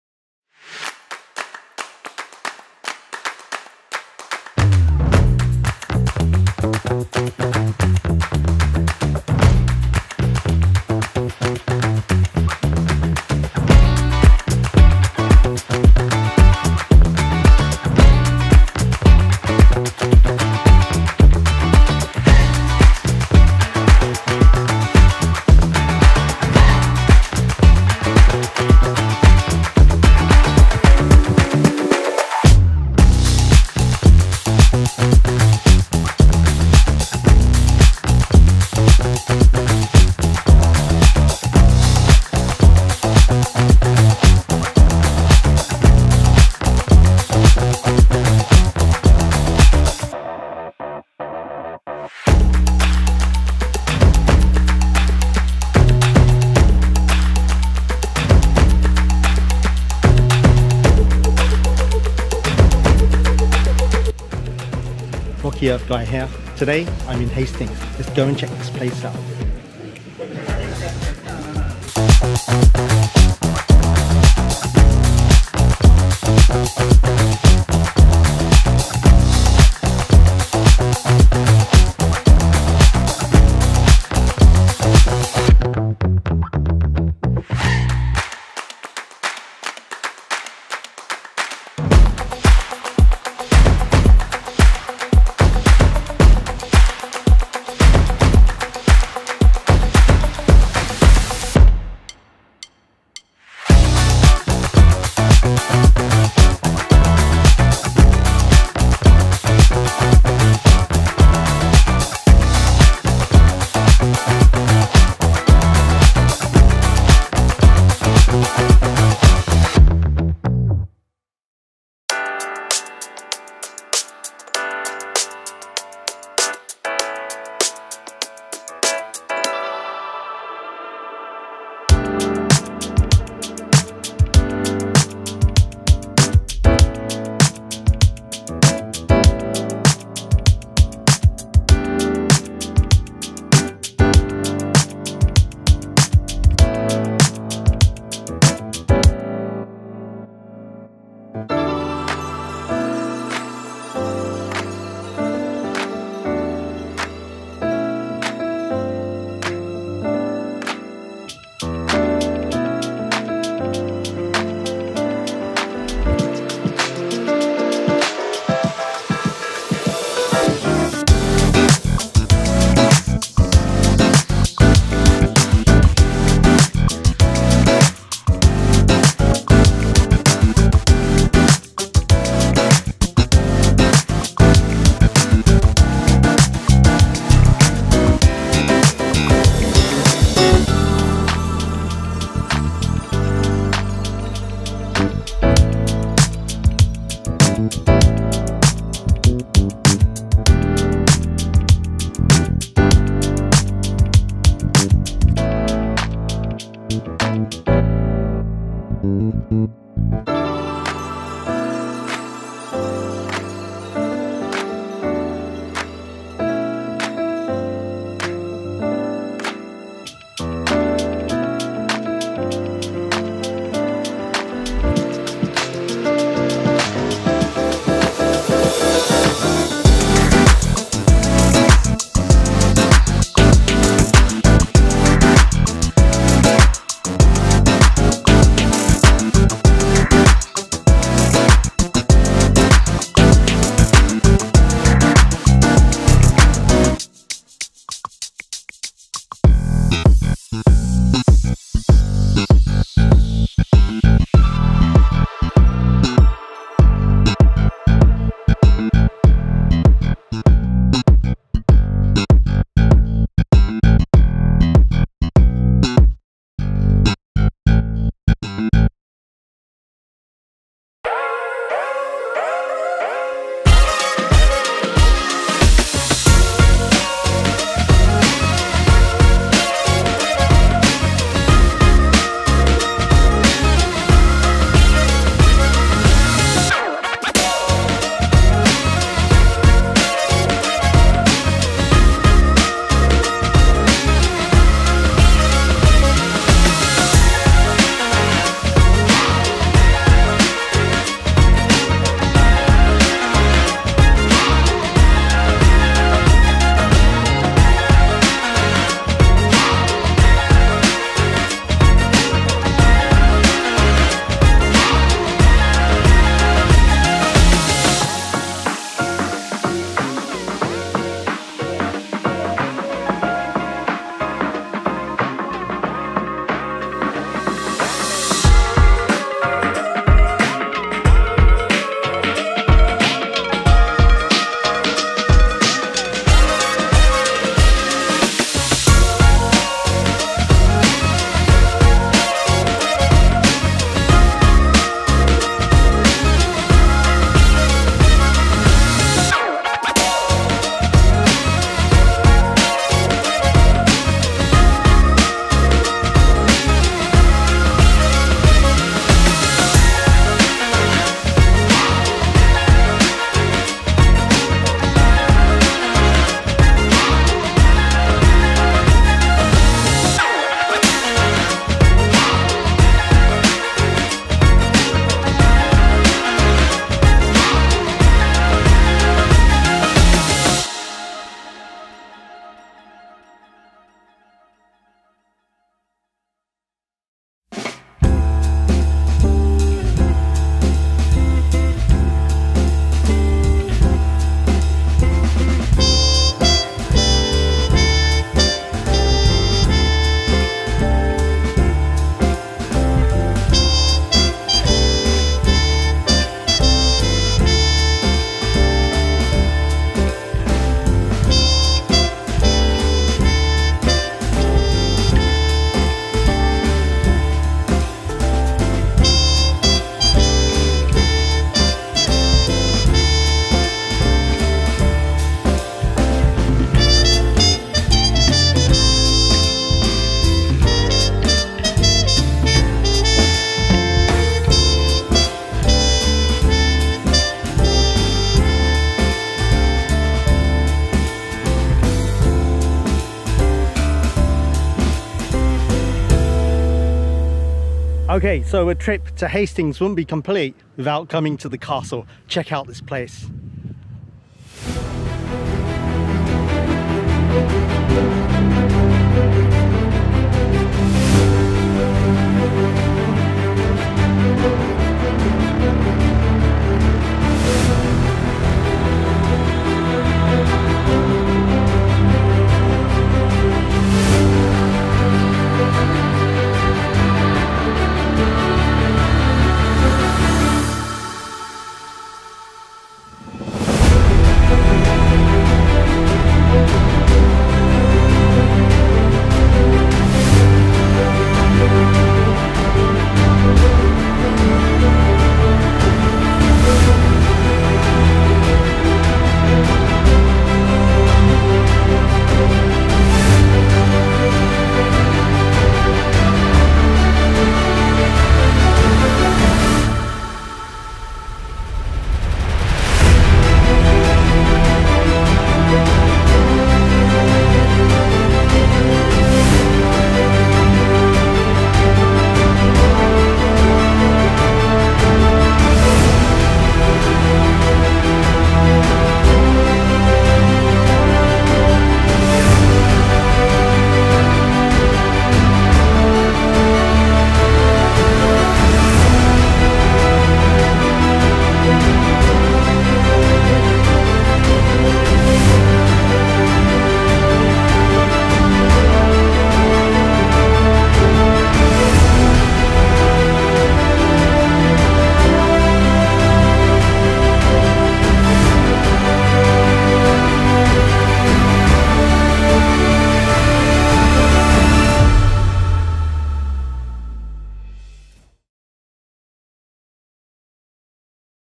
Exploring the Stunning Hastings Old Town Beach & Castle | Must-See Sights in England
Experience the picturesque Hastings Old Town Beach & Castle for yourself! Just a short walk from Hastings Town Centre, this amazing landmark has been preserved for centuries and has a wealth of history to offer. Take a stroll through the Old Town and learn about the charming architecture of the past. Then relax on the sandy beach and find out what locals love about their favorite spot by the sea. Discover why Hastings Old Town Beach & Castle is the perfect spot for a day out with friends or family. Visit with us and explore the history behind this beloved spot in Hastings!